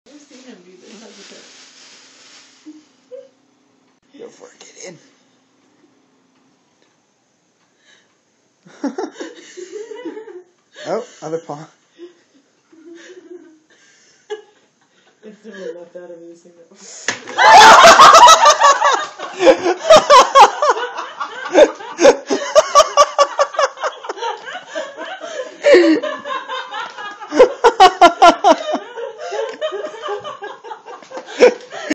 Cat vs gyrocopter
After already scaring the cat a couple times, Gideon decided to confront the small helicopter. Lets just say the small piece of plastic came out on top...